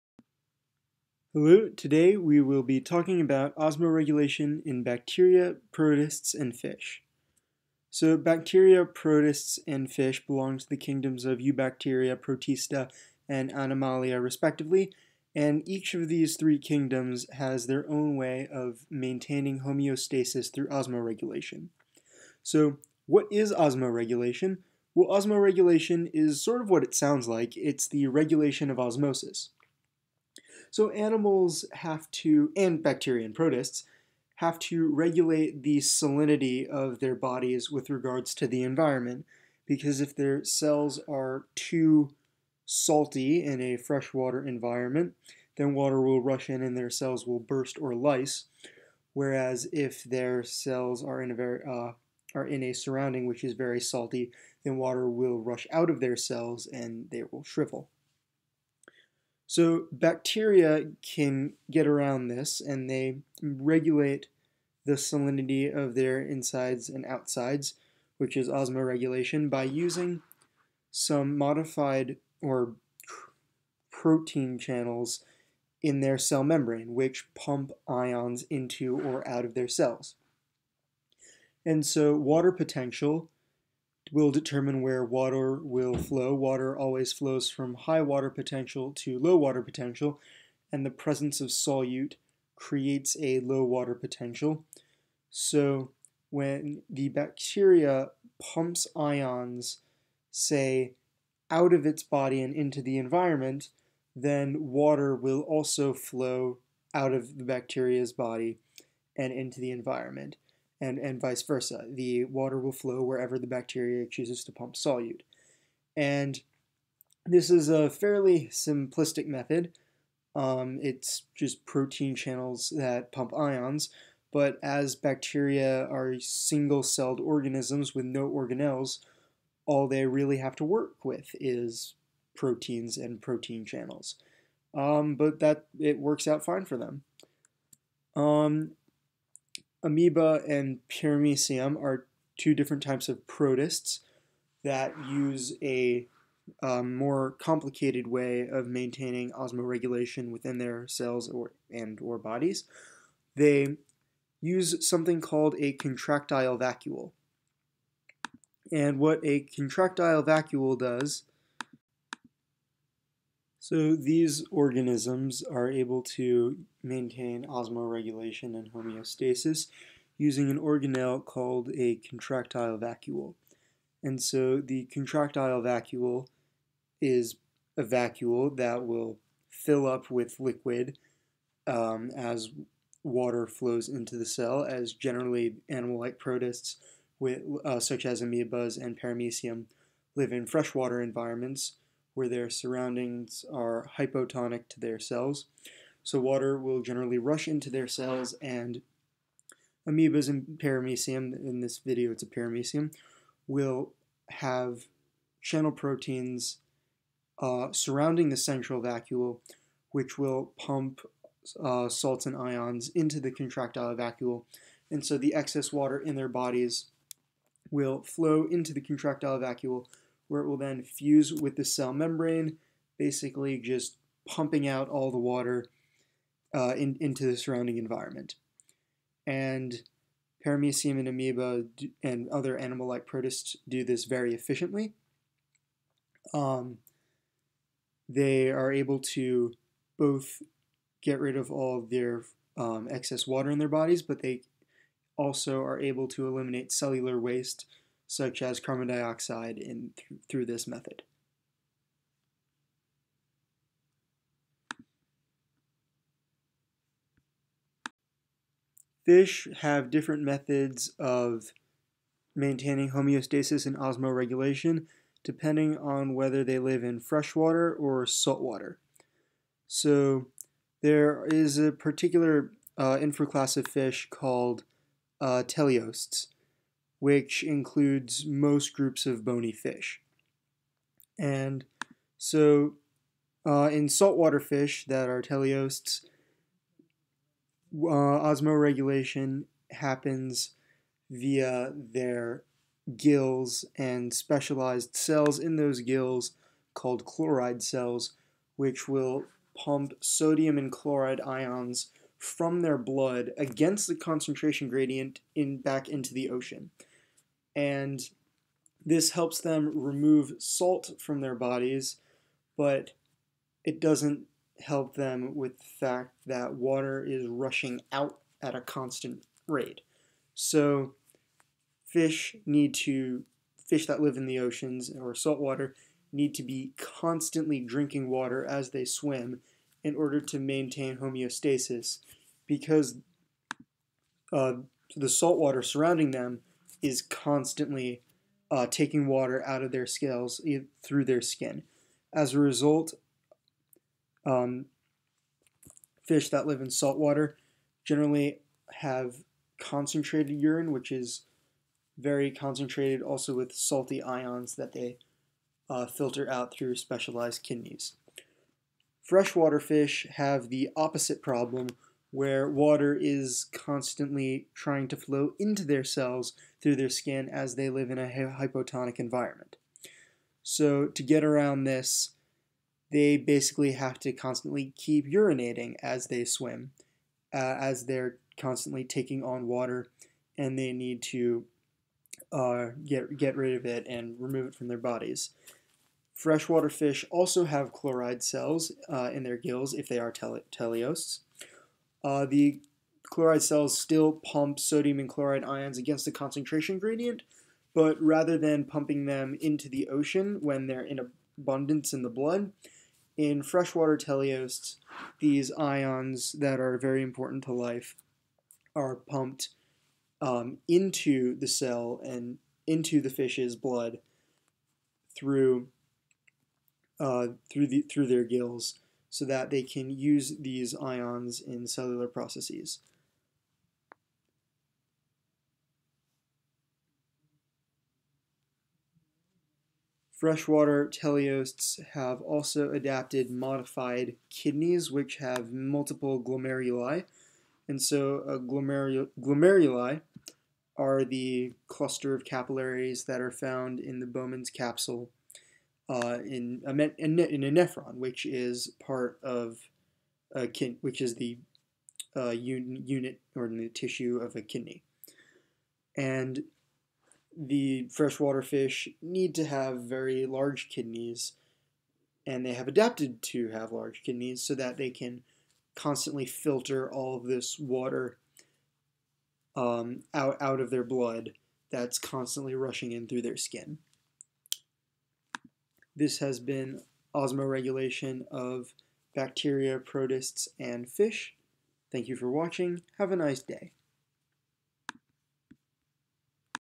Osmoregulation in Bacteria, Protists and Fish
This is a video I made for my AP Biology class on osmoregulation in different organisms.